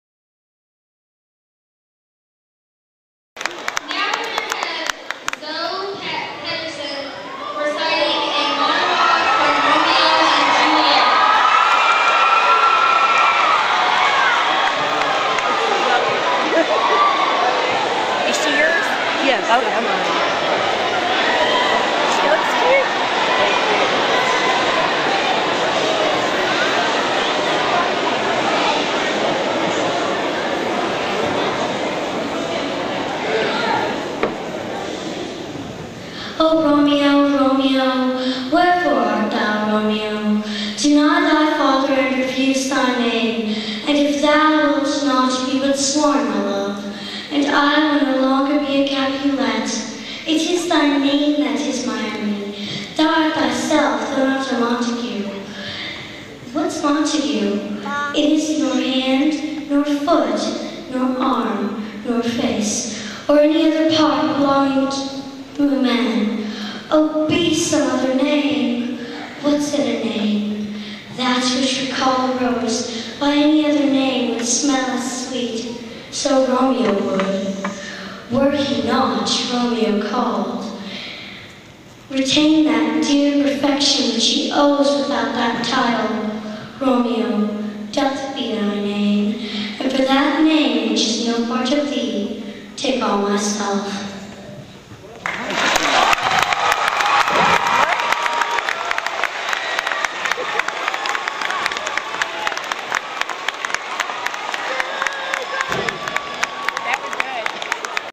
Zoe's Talent Show Monologue
3rd grader Zoe performs a monologue from 'Romeo & Juliet' in the school talent show.